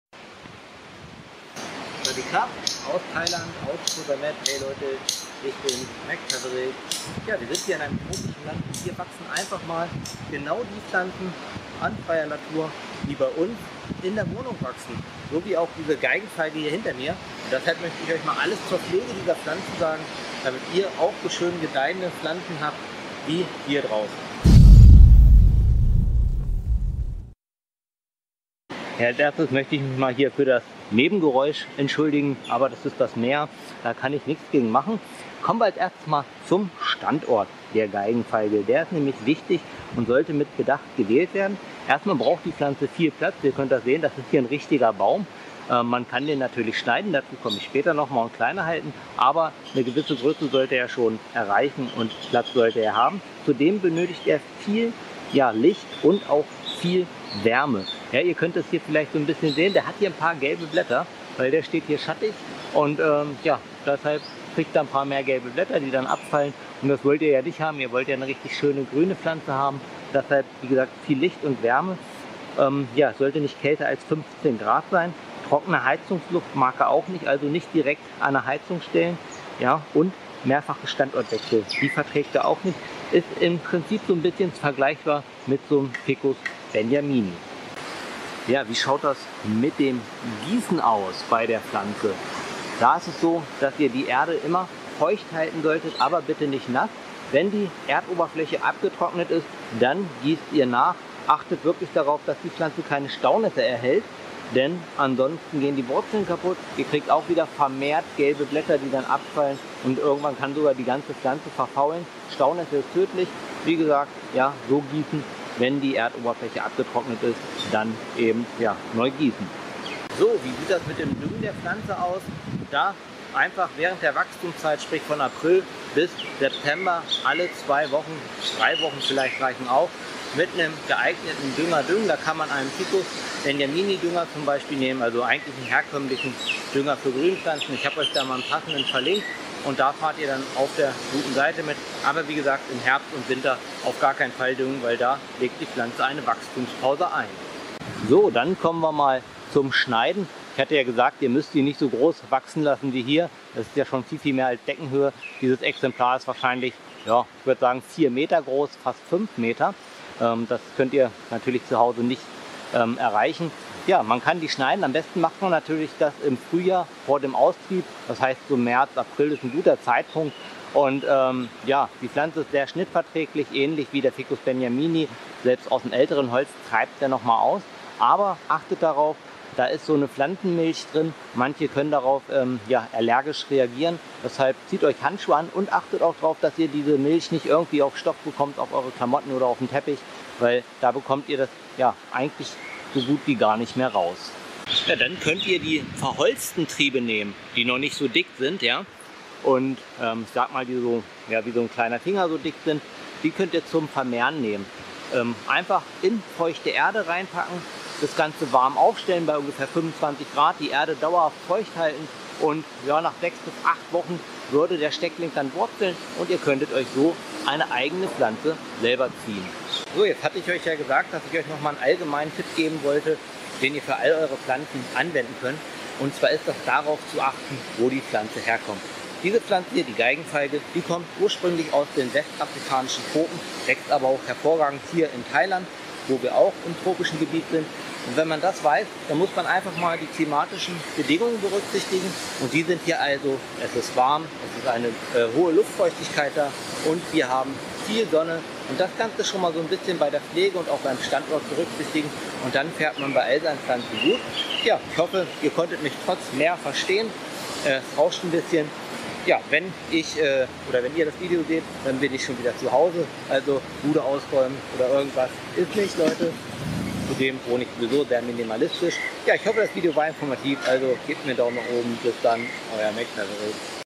Geigenfeige pflegen - Ficus lyrata Pflege, Düngen, Gießen, Standort, Schneiden, Vermehren uvm.
In diesem YouTube Video zeige ich euch alles zur Pflege einer Geigenfeige Pflanze. Ich zeige euch den Standort, das Gießen, Düngen, die Überwinterung uvm. Geigenfeige empfohlener Dünger

!!! Wer bis hierhin gelesen hat, schreibt in die Kommentare Streber ;)